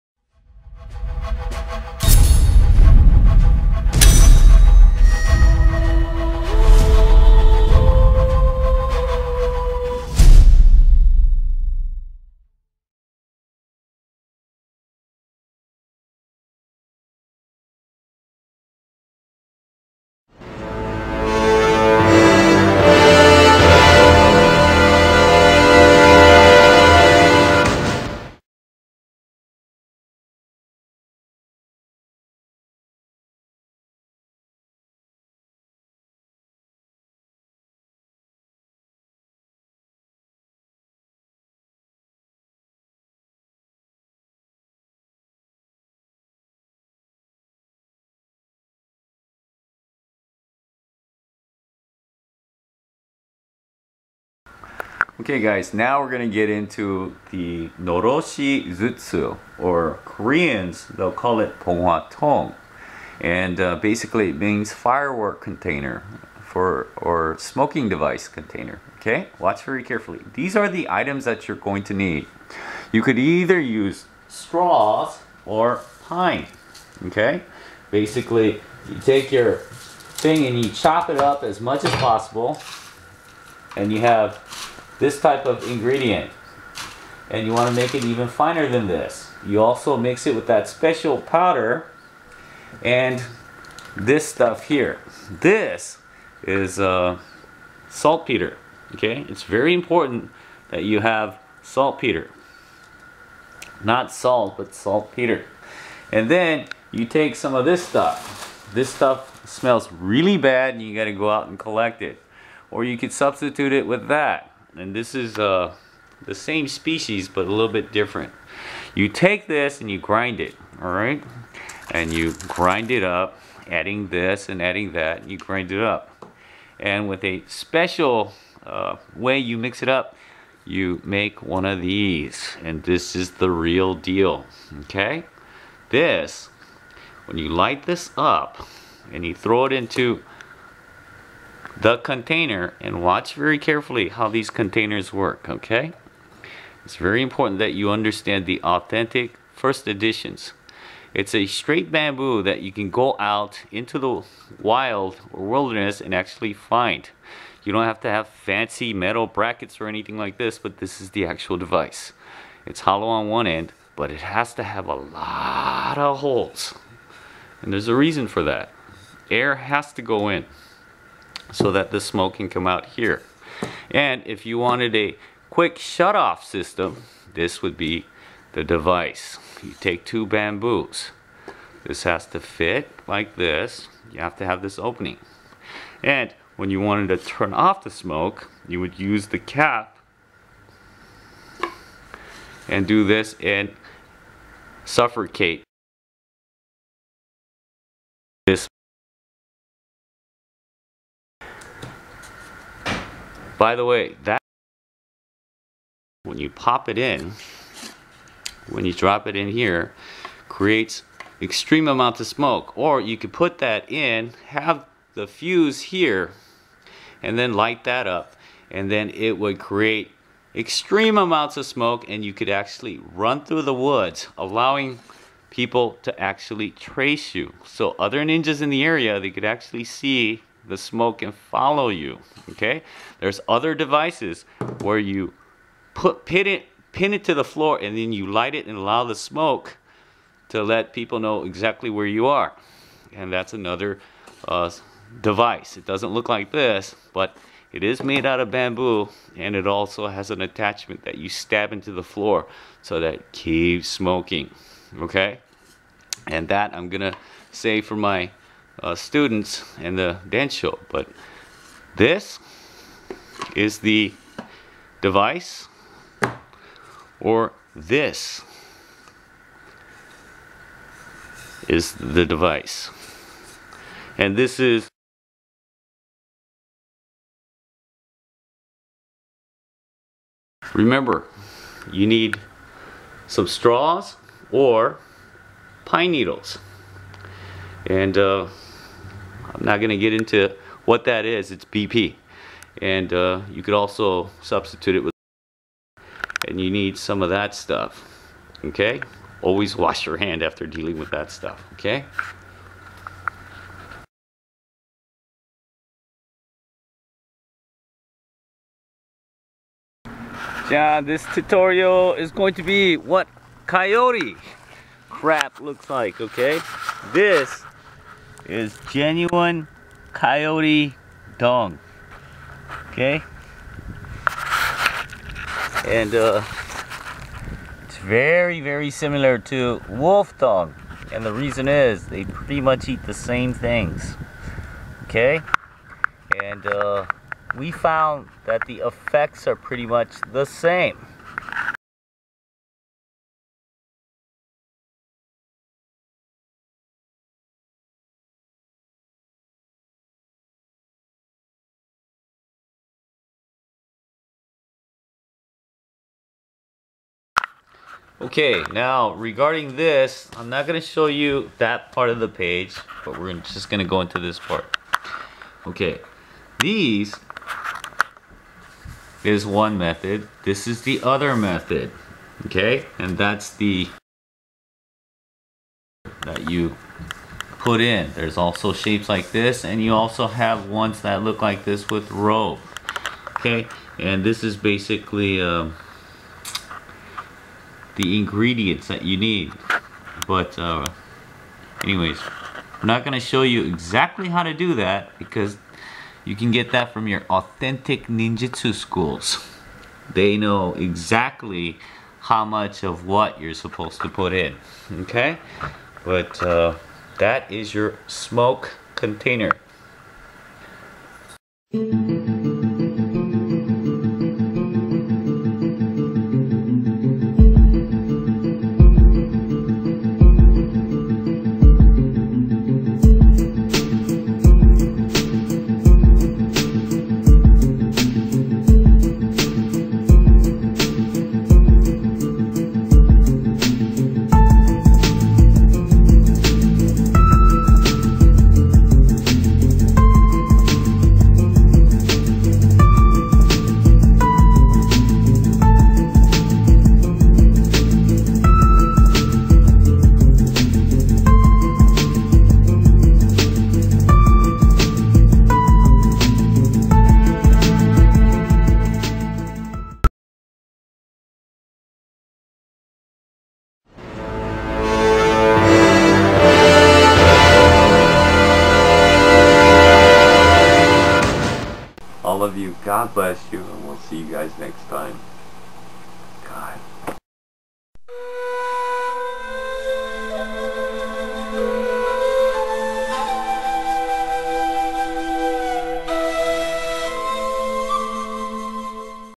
Noroshizutsu 狼煙筒( Authentic Ninjutsu 忍者 間者 Ninja tools)
WARNING: You may accidentally learn something for FREE if you continue to watch my videos!

Did you really think I don't know how to make authentic tools? These came from China to Korea to Japan and anyone who is authentic knows how to make and prepare them. Otherwise, who is the poser and fake? As for the Museum container, it's just to hold powder and droppings...those are nothing special.